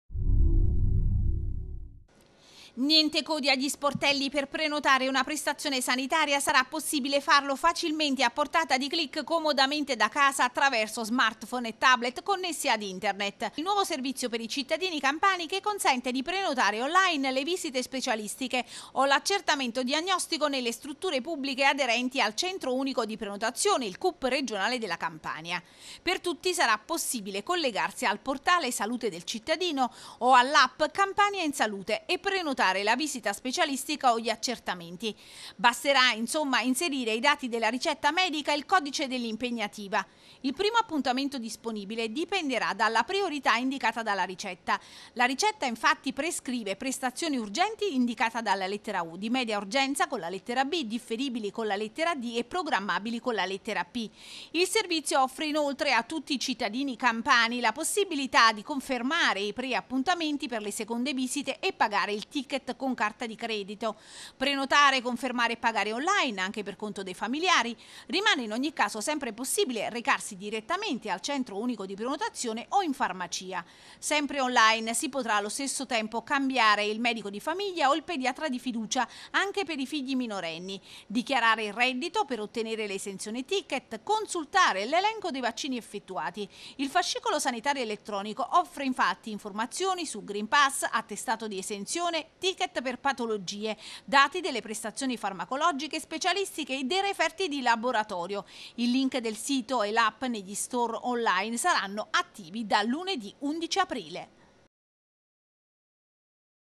Campania, Dall’11 aprile disponibile una APP per prenotare le visite specialistiche
Parte oggi  lunedì 11 aprile  il nuovo servizio per i cittadini campani che consente di prenotare on-line le visite specialistiche. Per tutti sarà infatti possibile collegarsi al Portale Salute del Cittadino e alla App Campania in Salute, e prenotare la visita specialistica o l’accertamento diagnostico nelle strutture pubbliche aderenti al Centro Unico di Prenotazione Regionale della Campania. Basterà inserire i dati della ricetta medica.